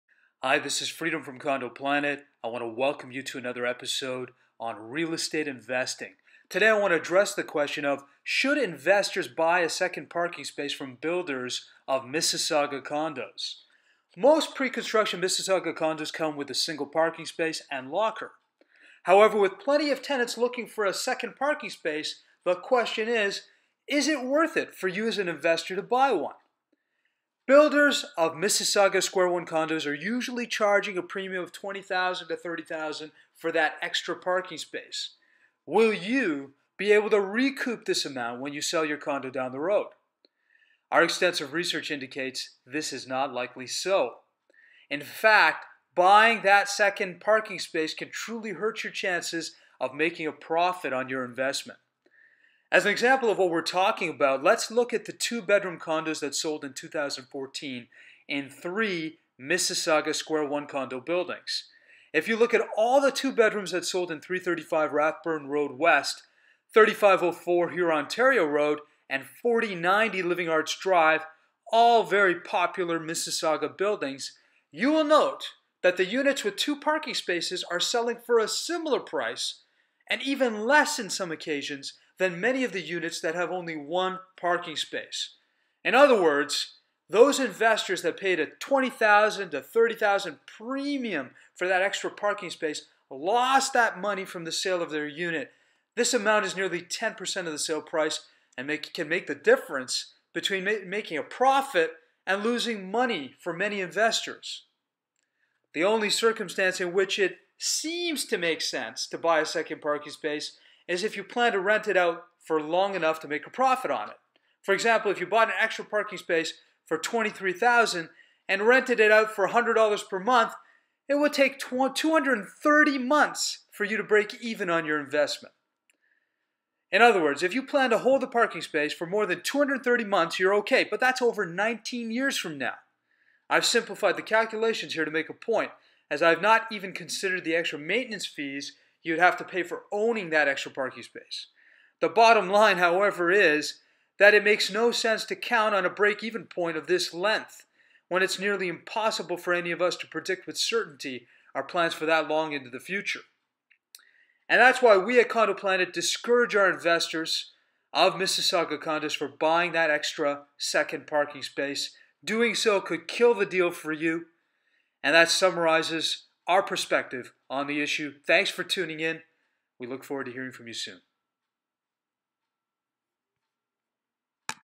Should Real Estate Investors Buy a 2nd Parking Space from Condo Builders
Book a Discovery Call with Me

This video explores whether investors should buy an extra parking space when they register to purchase a pre-construction condo.  Though there is a demand among tenants for a second parking space, does buying one make financial sense?  Will investors make a profit on such a purchase? View this video to find out.